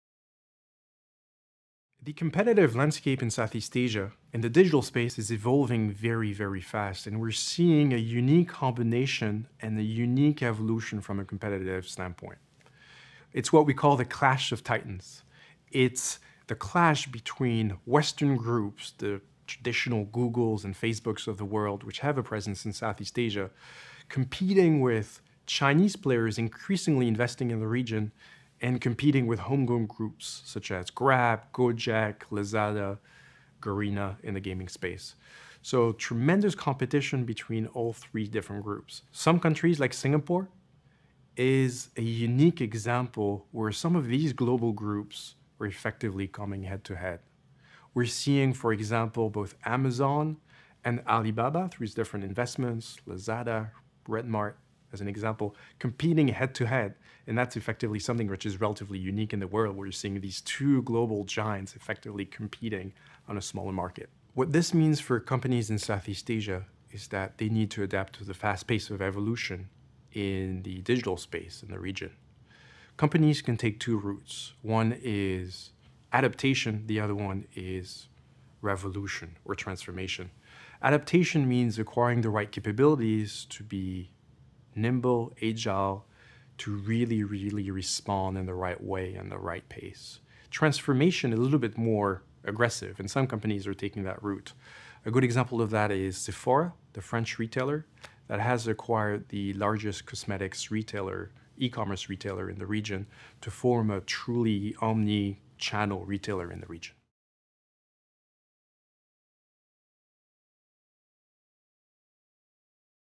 How to Win in Southeast Asia's E-commerce Market
Sebastien Lamy, a partner with Bain's Consumer Products practice, outlines two routes companies can take to thrive in this fast-moving market.